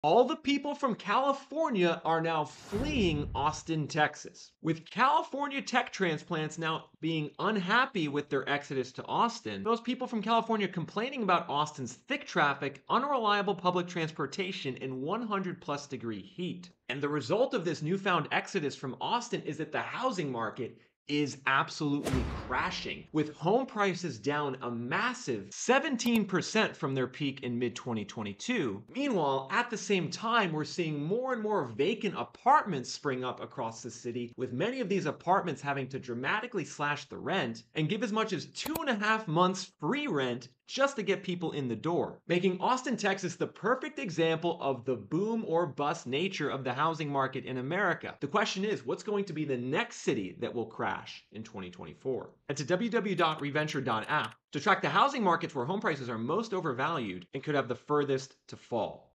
Californians are FLEEING Texas. Home prices crashing.
Californians are now fleeing cities like Austin, TX. And home prices and rental rates are crashing as a result.

Austin, Texas was a boomtown during the pandemic that many Californians moved to. However, now, many are moving back. And this has resulted in a 17% crash in home prices over the last two years, to go along with a massive surge in vacant apartments.

Austin is the perfect example of the boom/bust nature of the housing cycle. Which other cities could be next to crash?